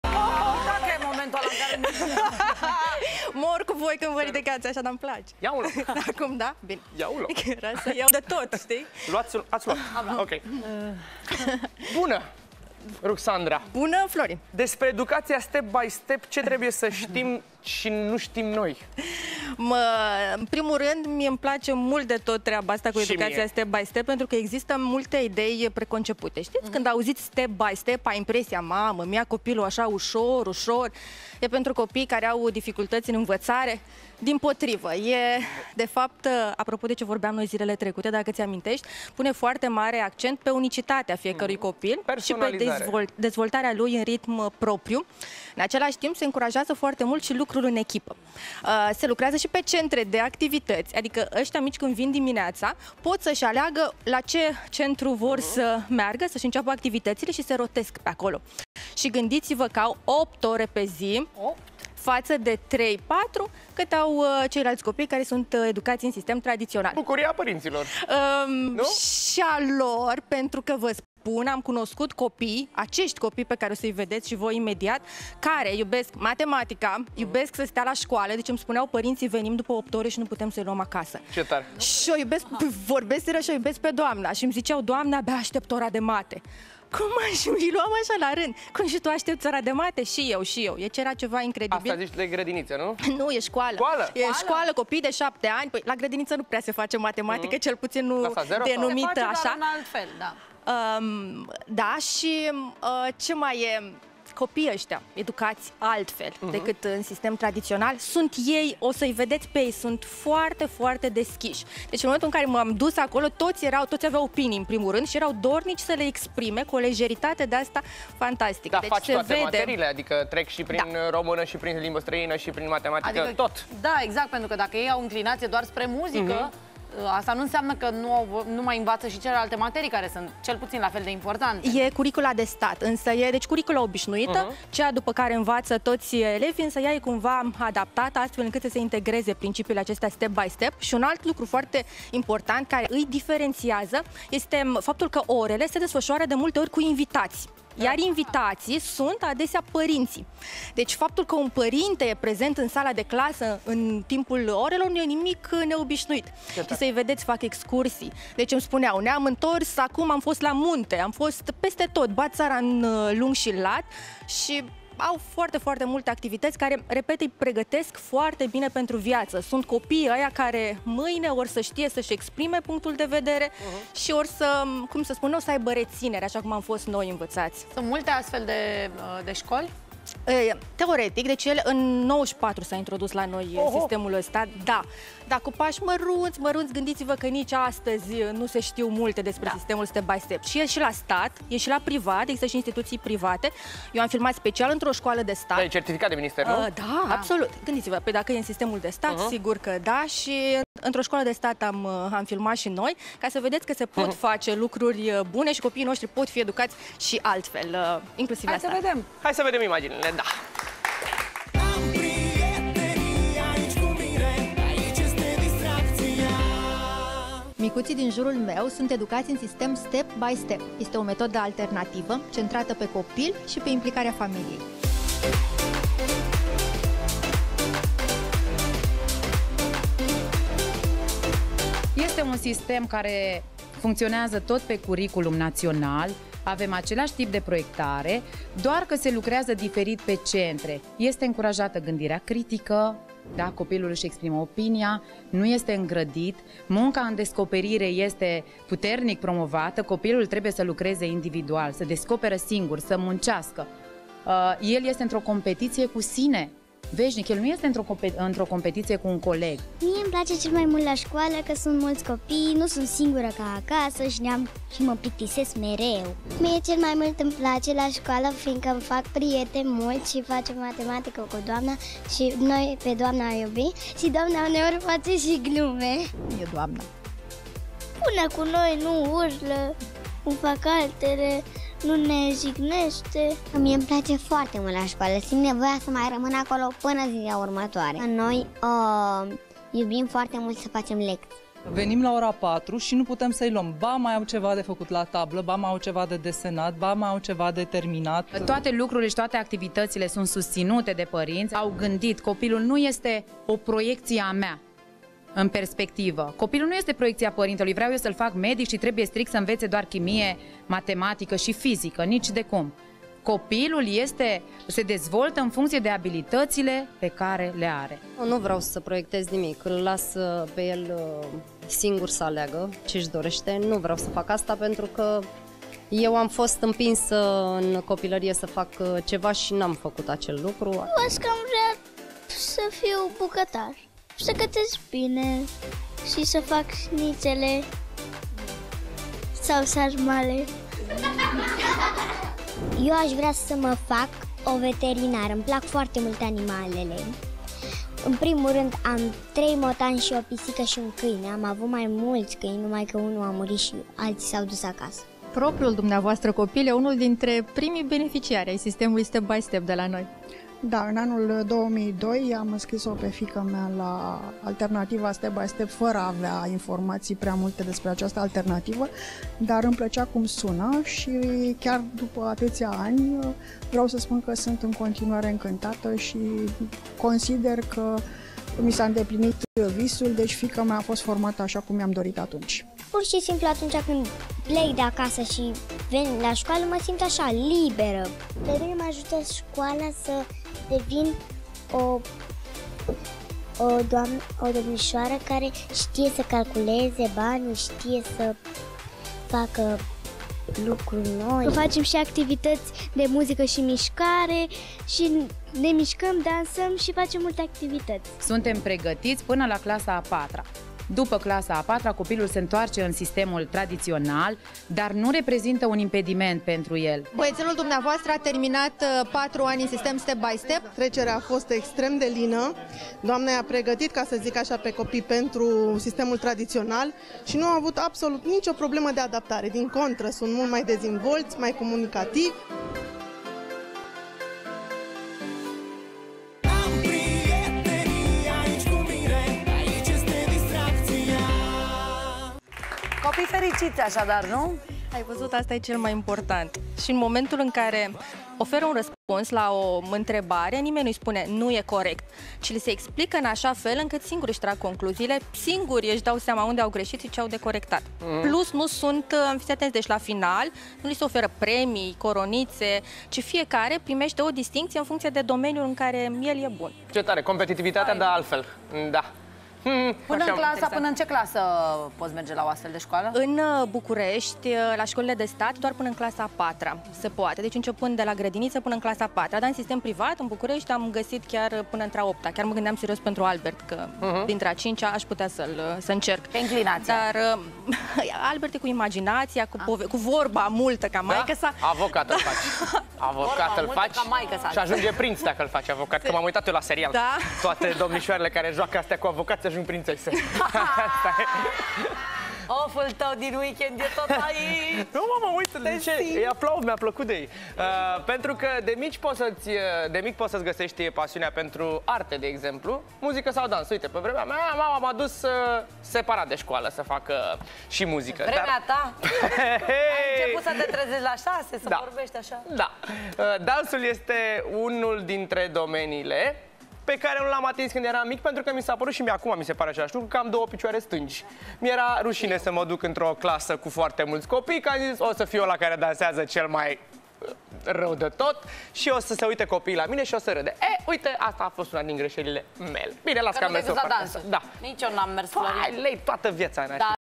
Ce trebuie să știm despre educația Step by Step
Step by Step este o metodă alternativă de educație care promovează metode de predare-învățare centrate pe copil și pe implicarea familiei.